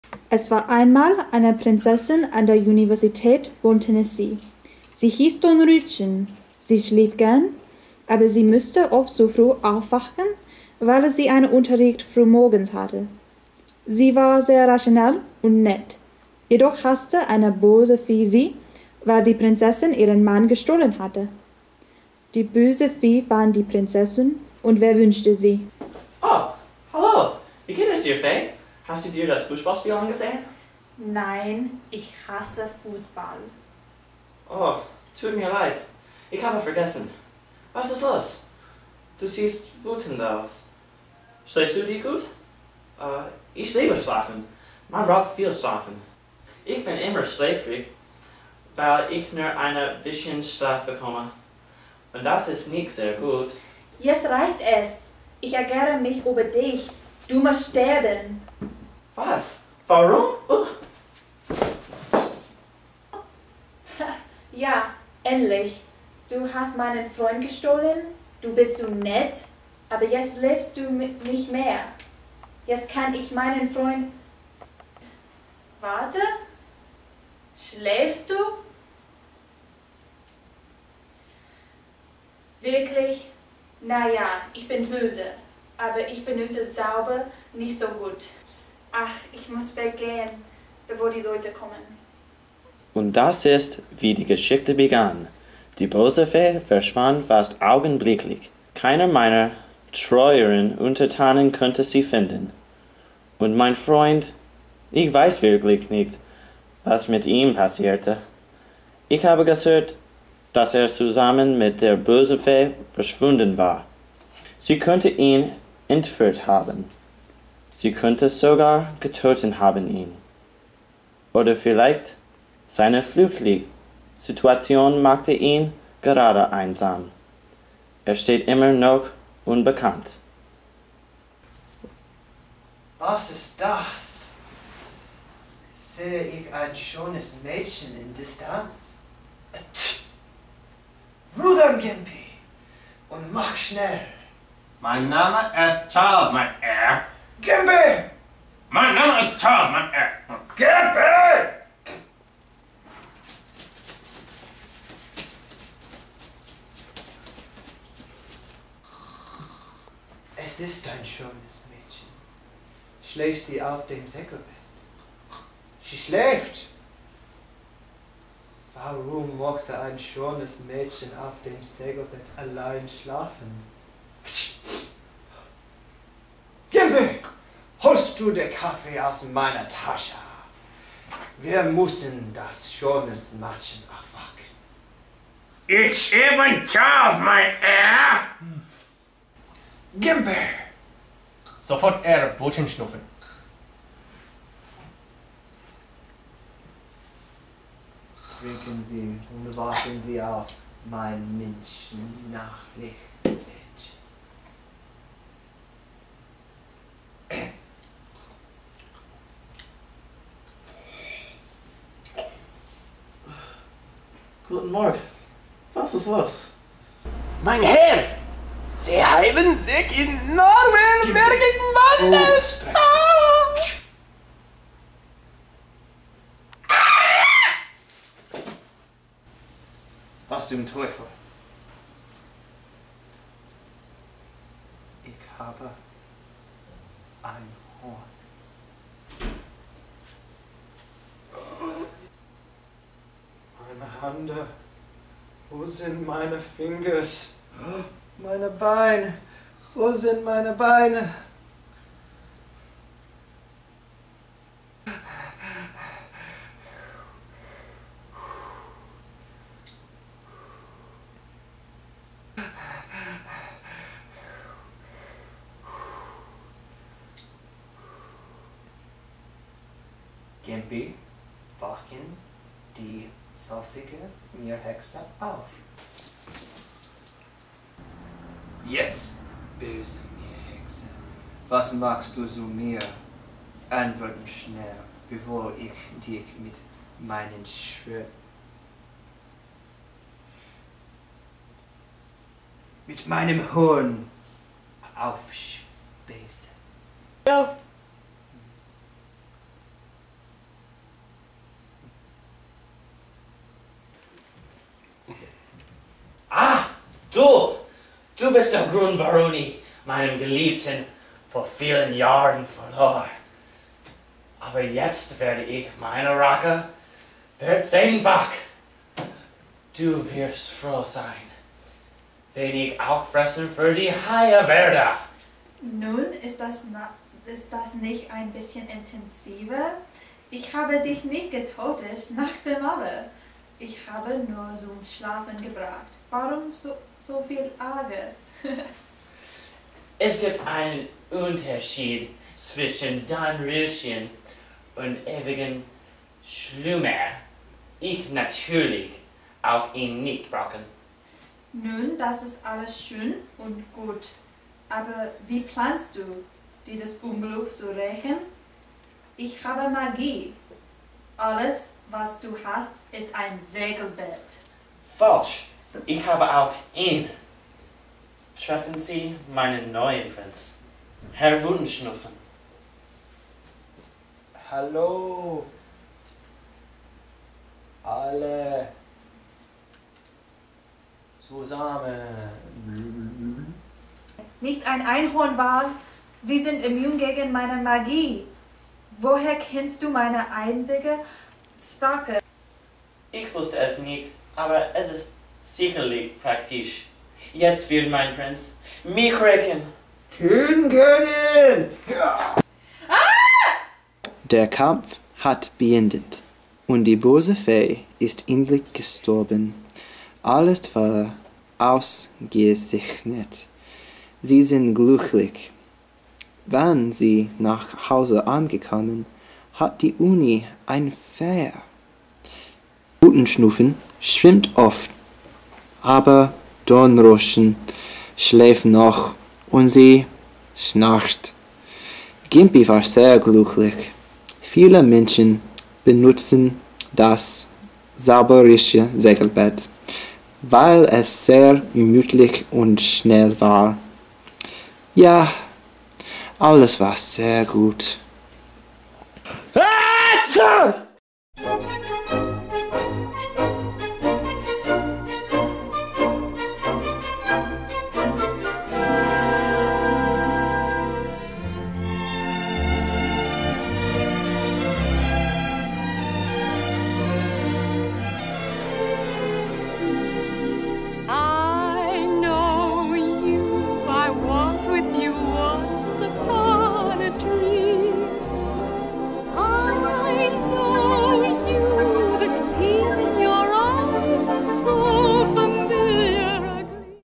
Dornroeschen.mov
Robert Tindell, Kelsey Ray, Kyle Walker's interpretation of Dornroeschen.